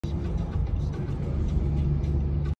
Seeing a Amazon semi truck on the road 6/26/24
I was on the road and I saw the truck so I took a video of it and I decided to record it because I hadn’t done a semi truck video in a while so enjoy